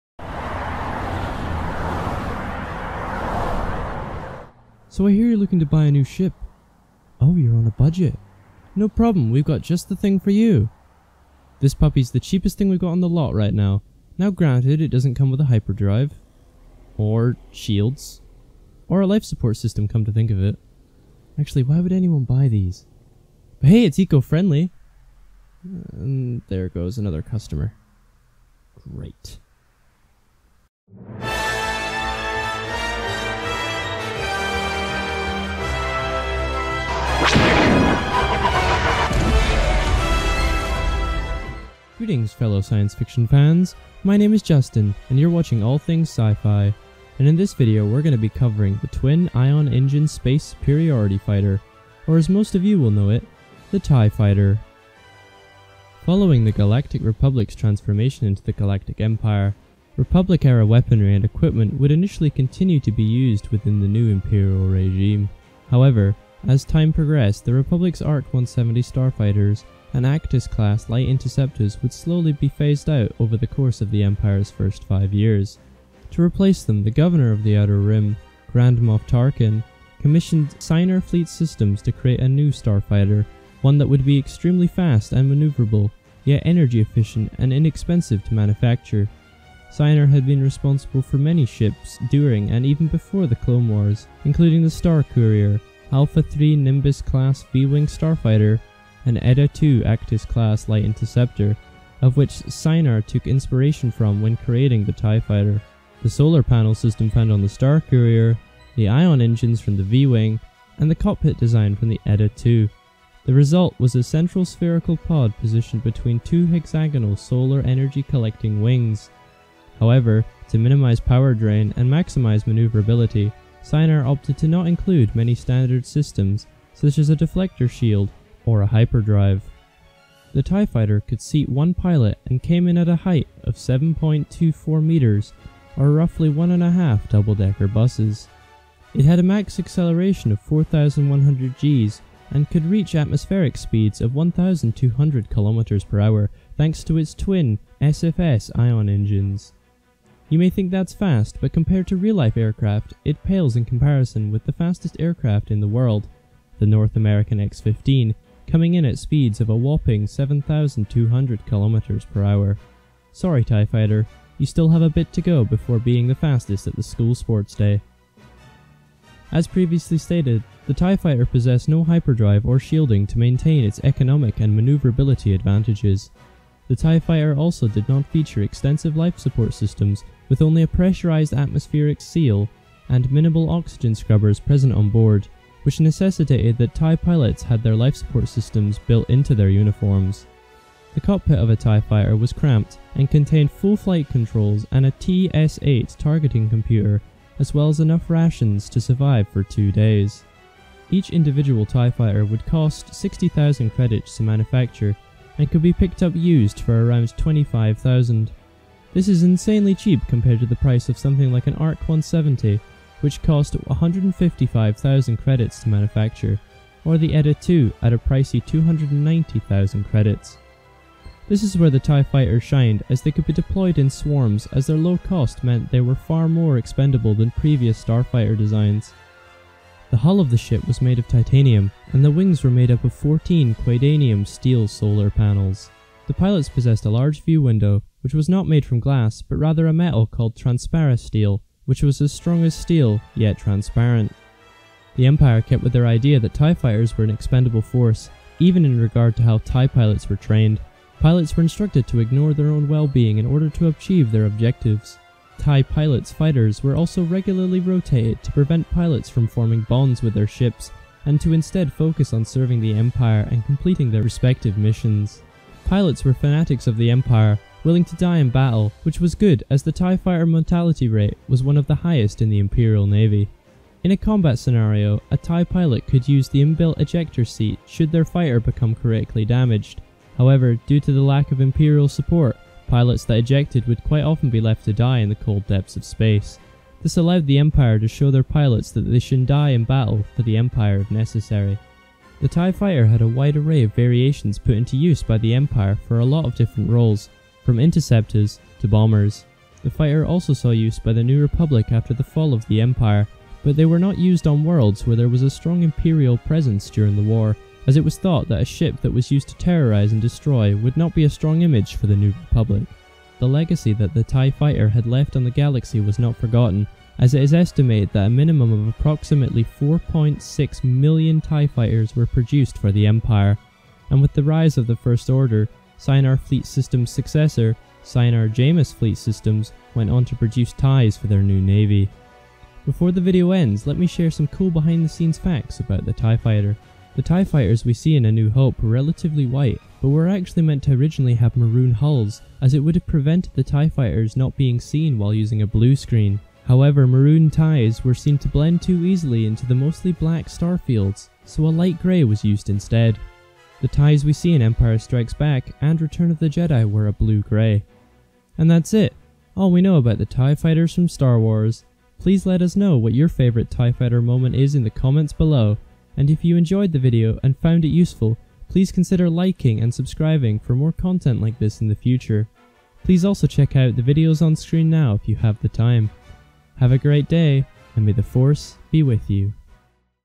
Star Wars: TIE Fighter Lore and History
This video is covering the Twin Ion Engine Starfighter or TIE fighter for short from Star Wars!
The TIE fighter was a terrifying symbol used by the Empire during their rule of the Galaxy, known for their horrifying screech the TIE fighters are definitely a fan favorite ship in Star Wars.